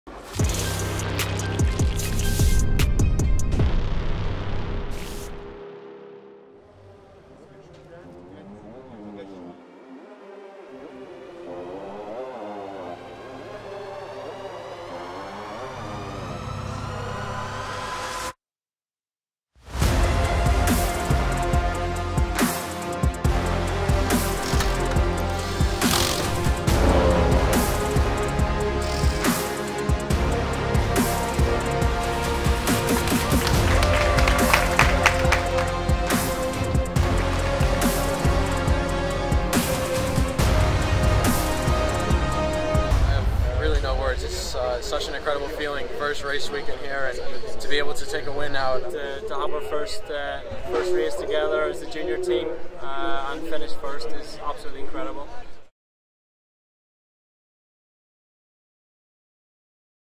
BMW Junior Team – First Win.
It is an indescribable feeling to conquer hell and to cross the finish line first. Experience pure joy after the first victory of the BMWJuniorTeam at the Nürburgring.